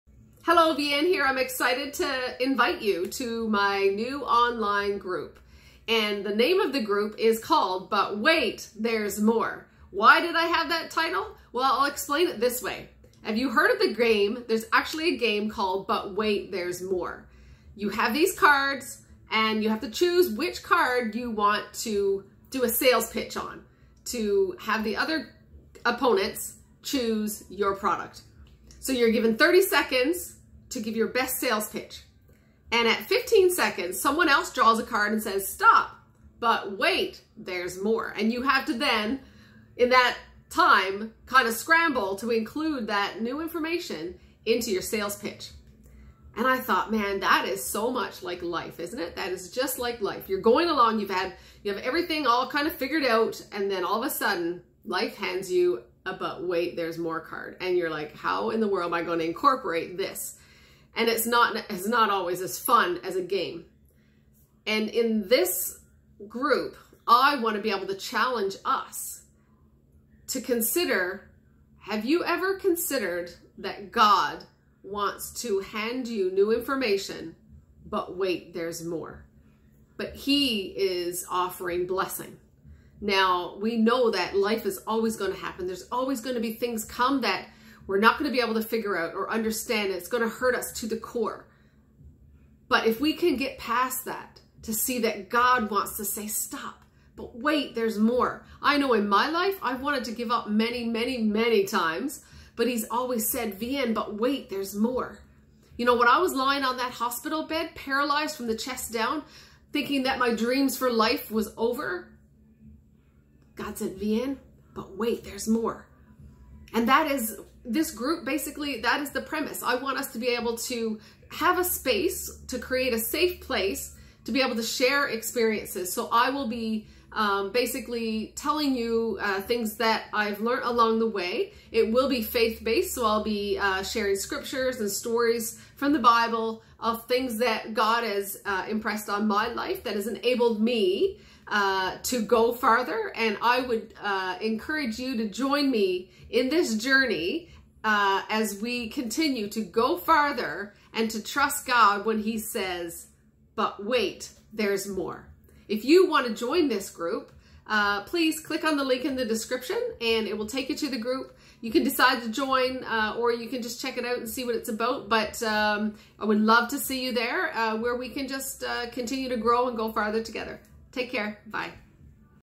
But WAIT There's MORE! WELCOME
How many of us have ever YELLED AT GOD, saying I'M DONE?
TRUST ME I HAVE TO.
BUT EACH TIME, God would say hang in there Vahen.
BUT WAIT THERE'S MORE!
Do you want MORE for YOUR life?
God is able to do exceedingly abundantly above ALL we could ask or think!
Ephesians 3:20
I have created this group "BUT WAIT THERE'S MORE" to invite you to join me in that adventure to discover God's intended MORE for YOU.
WHEN: EVERY Saturday
TIME: 9:00 am MST